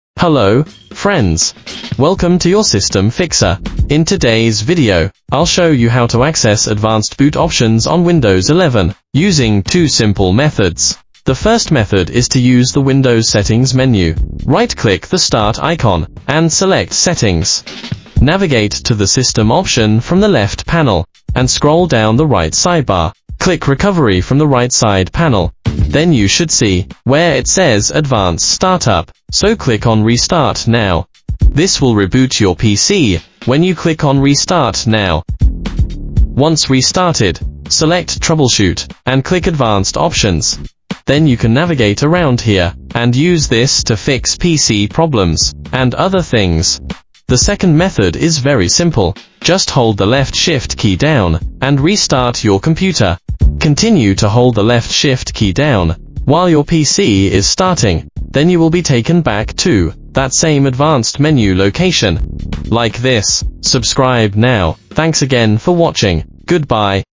2 Simple Ways to Access Advanced Boot Options on Windows 11
This video guides you through 2 simple methods to access Advanced Startup Options (WinRE) on Windows 11. When would you need to boot into Advanced Boot Options? Probably to reset this PC or repair your computer. You can also gain access to Safe Mode, uninstall the latest Windows updates, and much more.